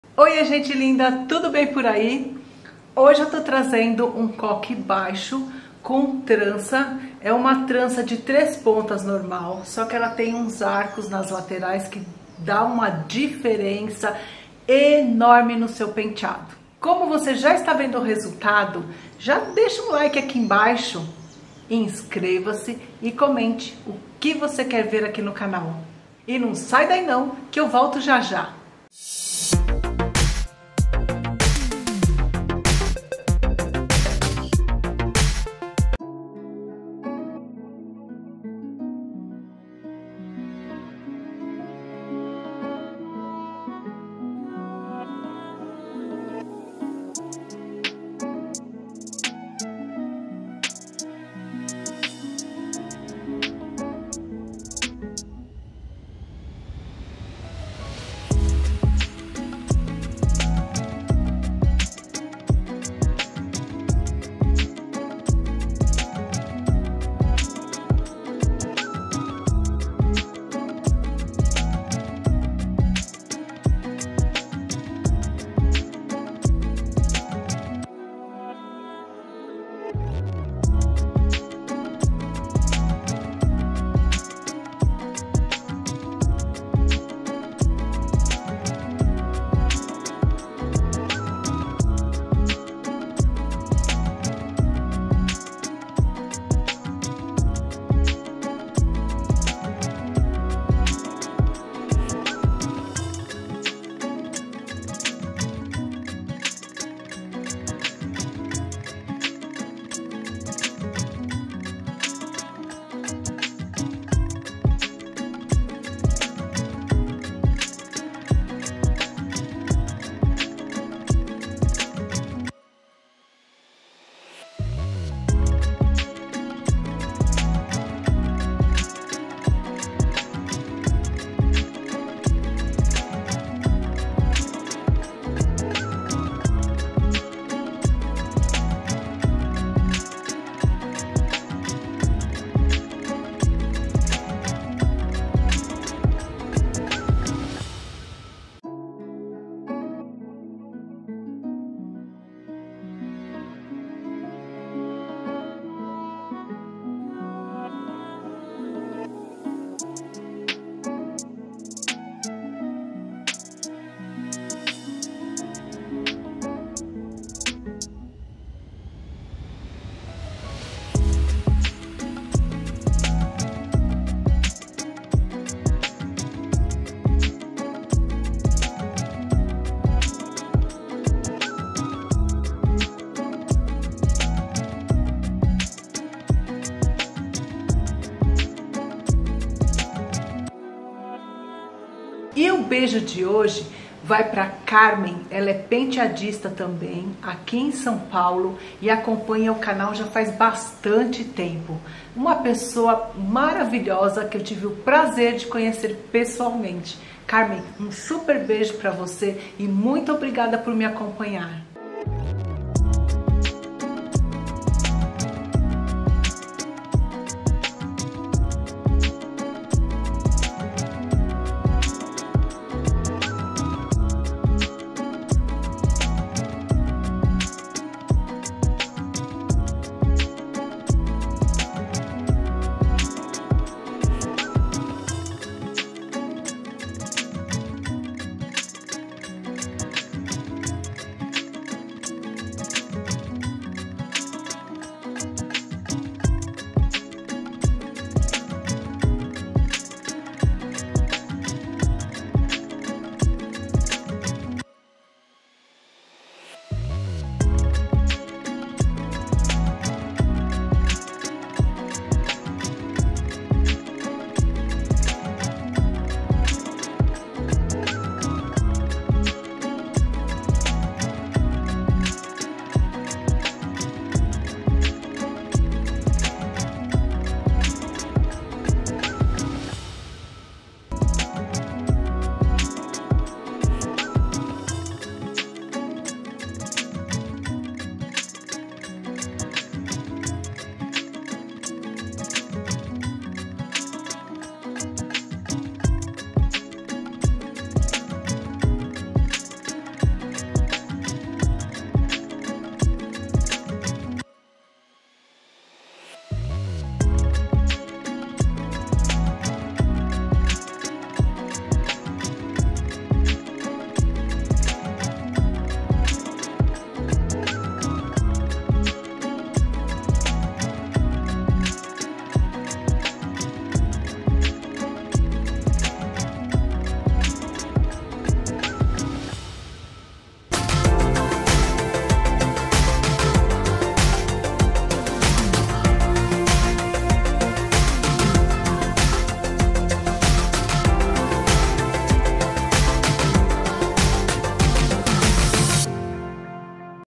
como fazer coque com trança
passo a passo de coque baixo com trança para noiva, madrinha, formanda, convidada.
paso a paso peinado de novia, madrina, invitada, graduación
bridal up do, wedding hairstyle